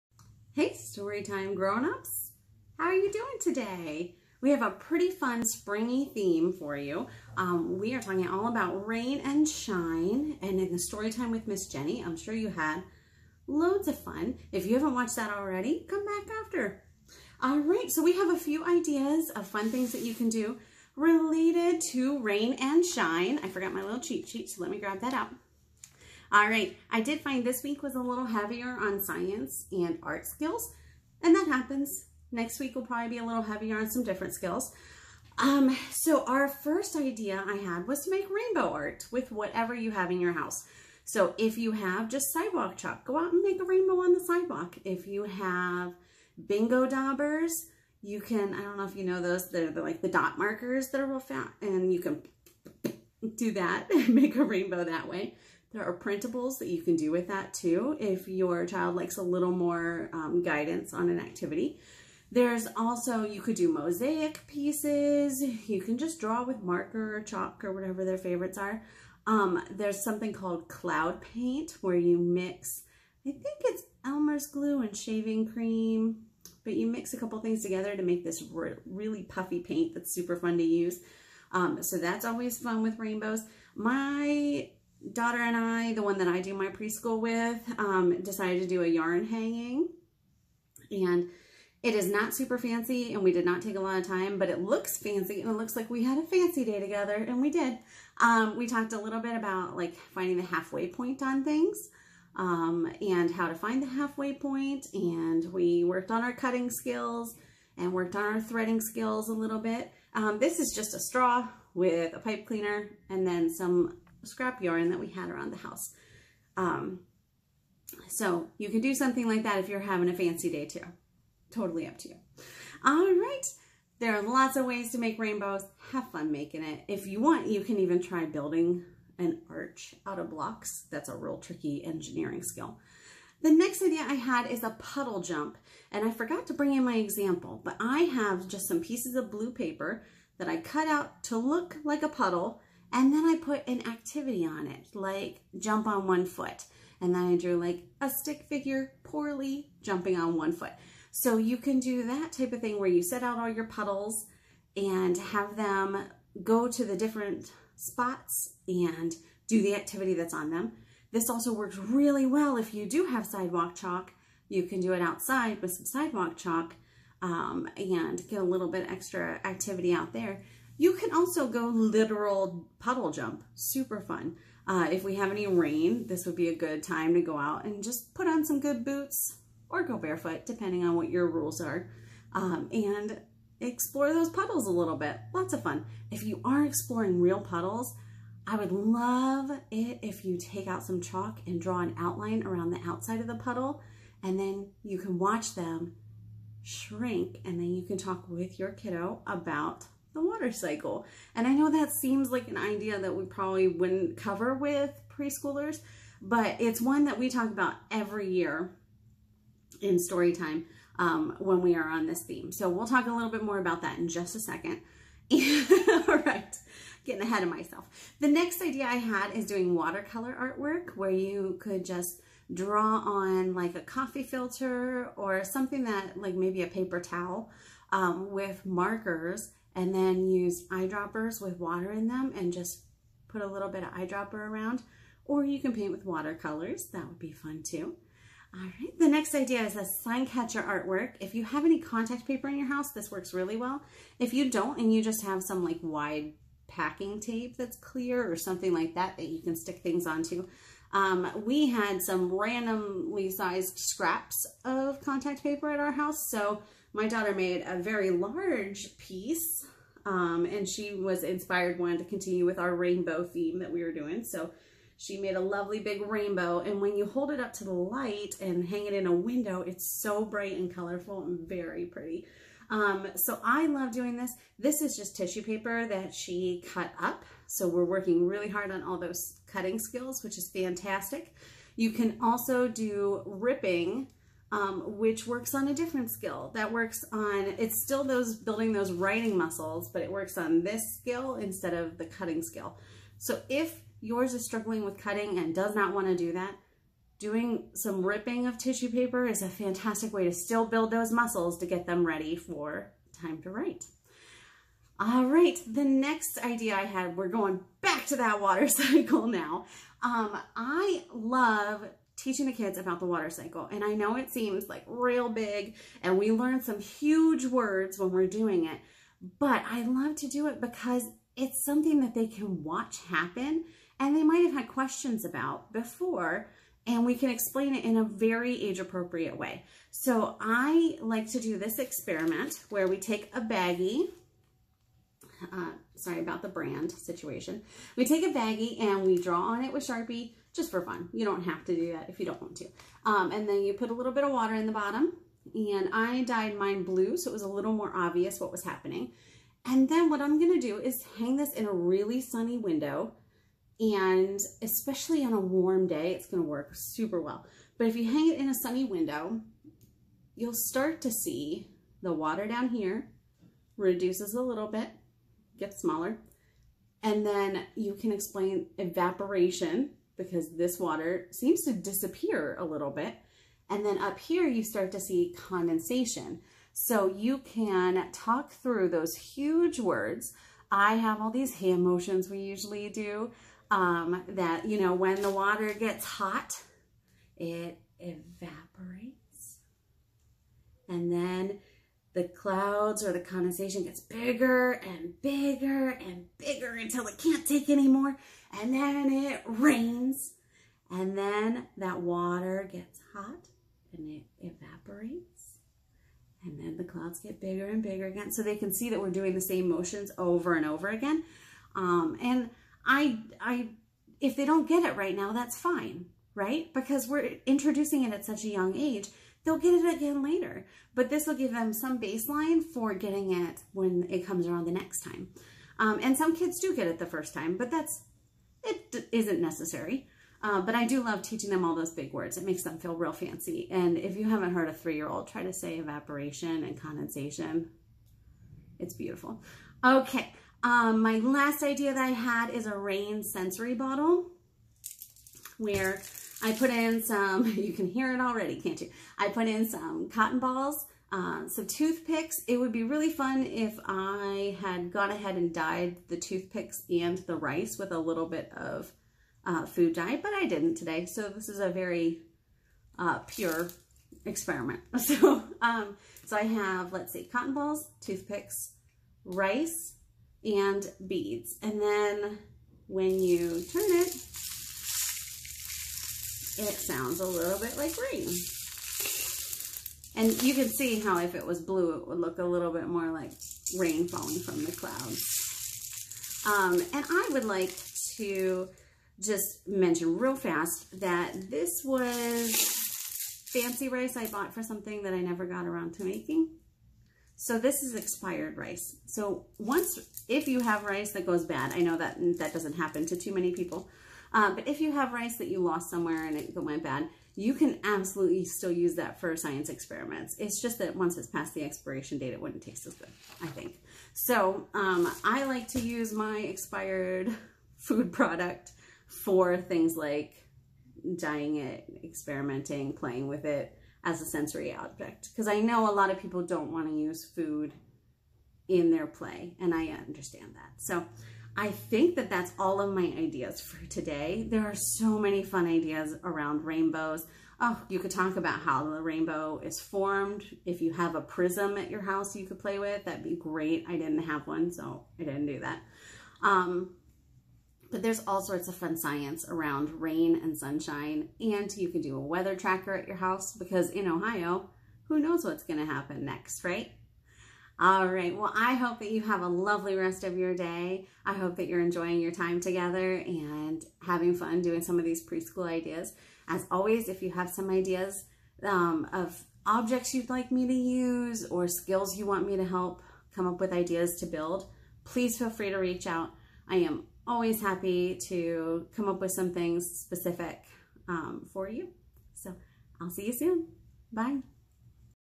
Extend the Storytime - Rain
Lisa, a former preschool teacher, offers lots of ideas for parents and caregivers looking for learning activities to accompany Miss Jenny's rain-themed storytime. Tested on her own preschooler, Lisa suggests creating rainbow art, a puddle jump game, a rain sensory bottle and much more!